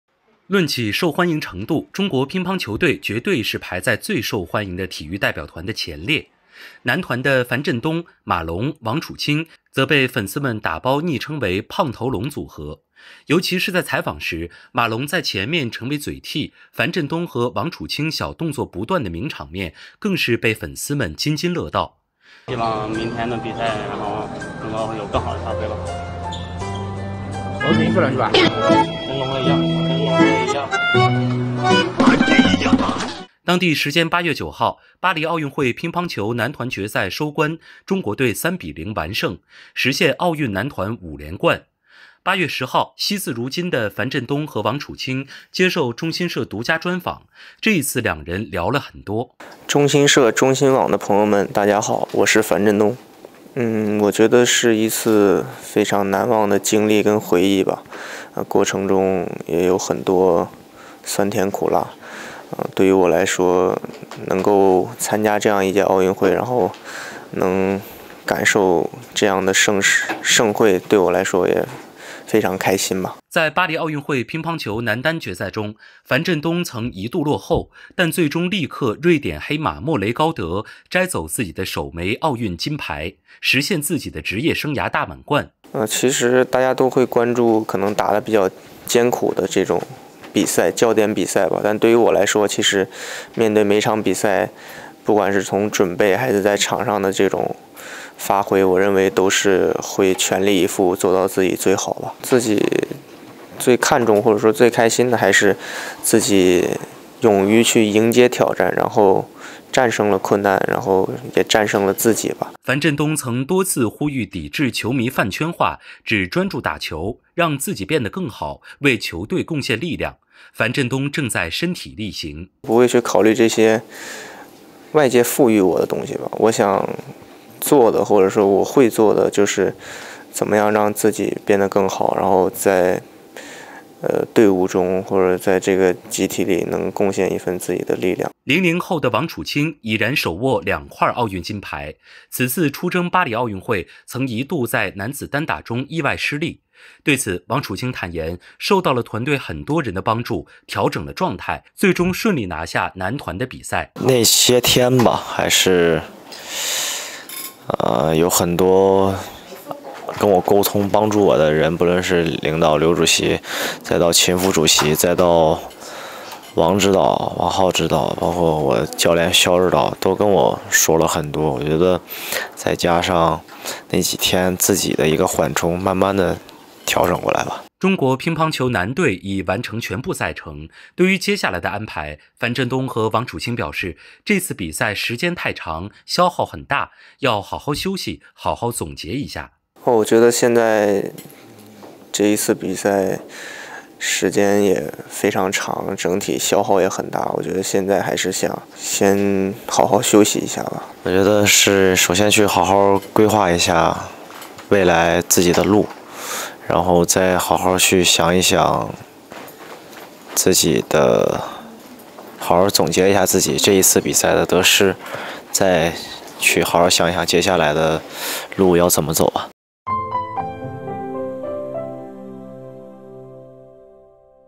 【巴黎奥运会】躲在“龙队”背后不说话？接受这家媒体采访樊振东王楚钦聊了很多
论起受欢迎程度，中国乒乓球队绝对是排在“最受欢迎的体育代表团”的前列。男团的樊振东、马龙、王楚钦，则被粉丝们打包昵称为“胖头龙”组合。尤其是在采访时，马龙在前面成为“嘴替”，樊振东和王楚钦小动作不断的名场面，更是被粉丝们津津乐道。

当地时间8月9日，巴黎奥运会乒乓球男团决赛收官，中国队3：0完胜，实现奥运男团五连冠。8月10日，惜字如金的樊振东和王楚钦，接受中新社独家专访，这一次，俩人聊了很多。